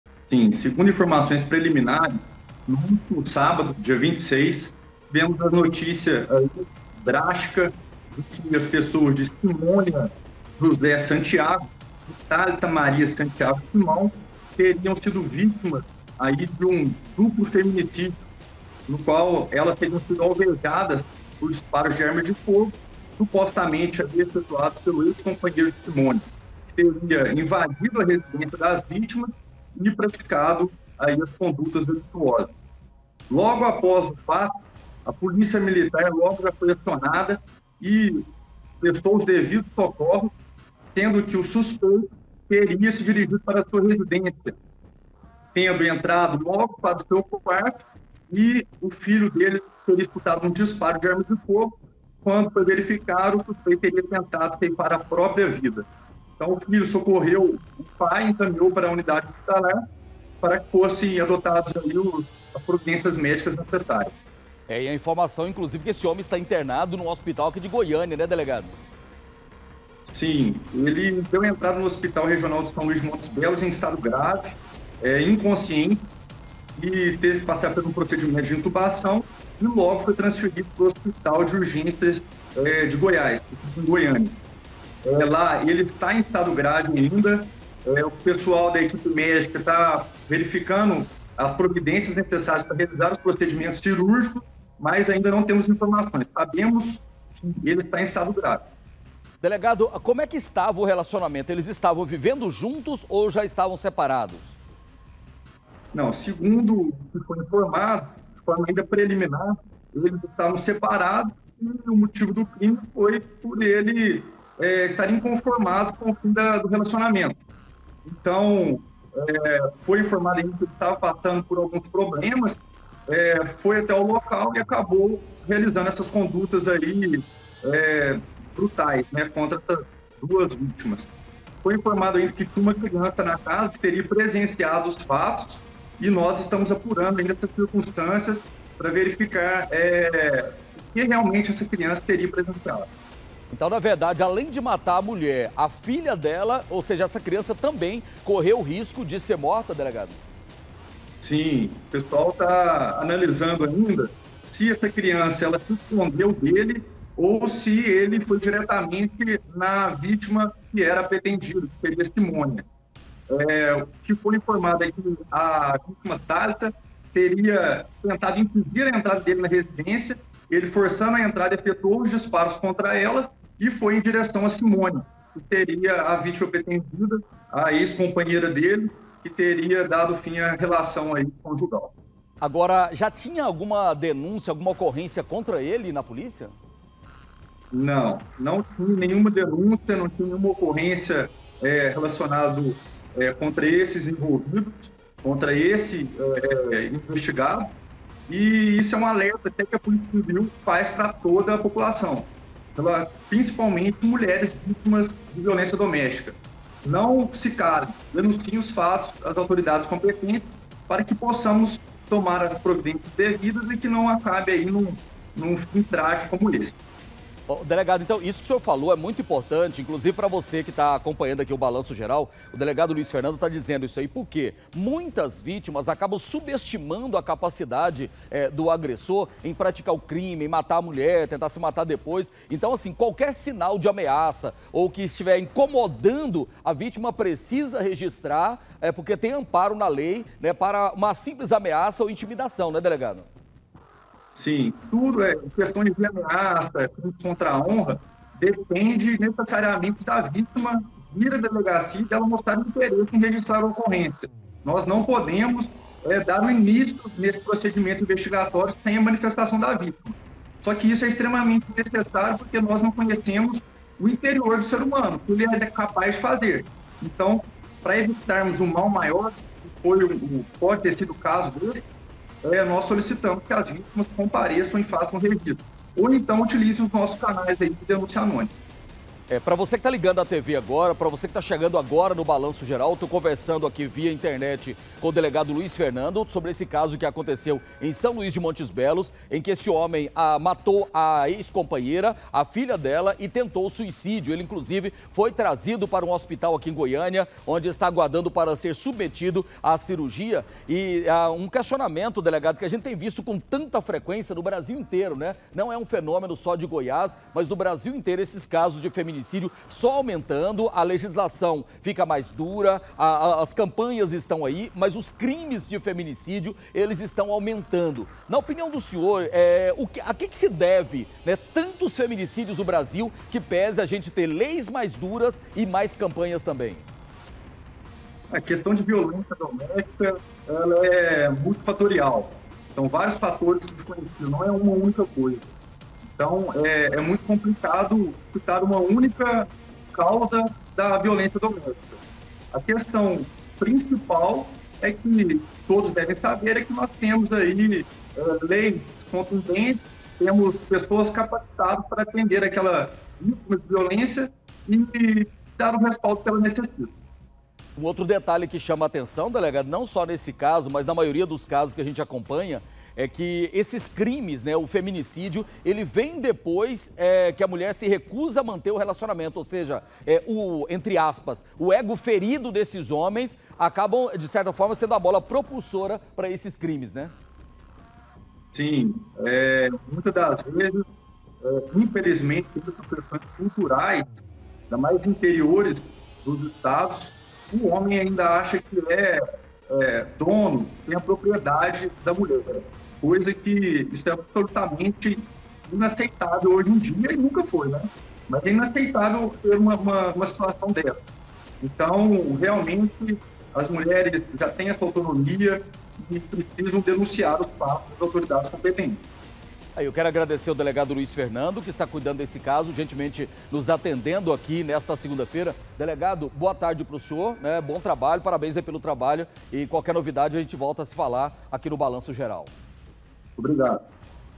FEMINICÍDIO: HOMEM NÃO ACEITA FIM DE RELACIONAMENTO E MATA MÃE E FILHA
O DELEGADO LUIZ FERNANDO ESTÁ POR SKYPE E TRÁS MAIS DETALHES SOBRE AS MORTES DE MÃE E FILHA EM SÃO LUÍS DE MONTES BELOS.